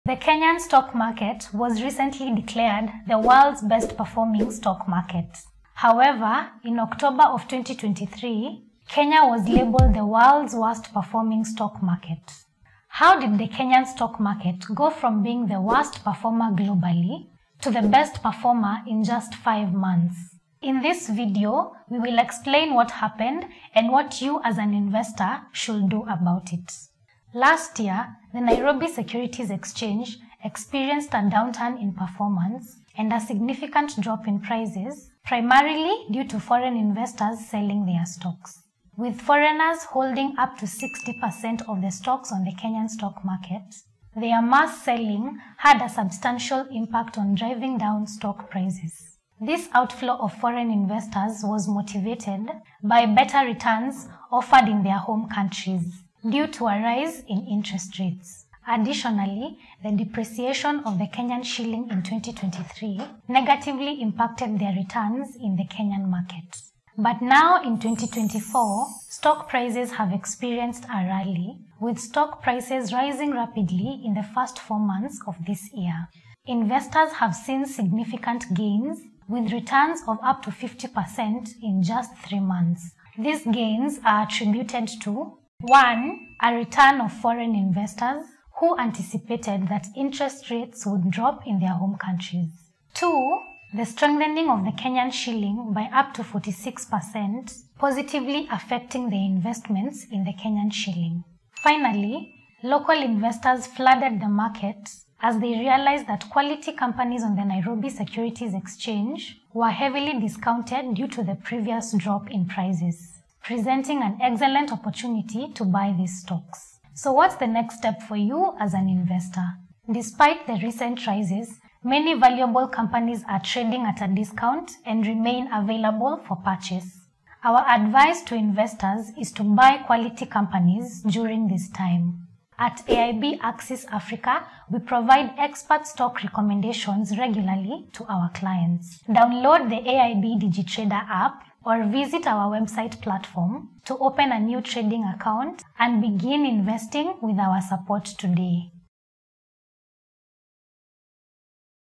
KENYAN STOCK MARKET RANKED BEST PERFORMING STOCK MARKET 💯🔥
Kenya's stock market was recently ranked the world's best performing stock market, Watch this video to find out why.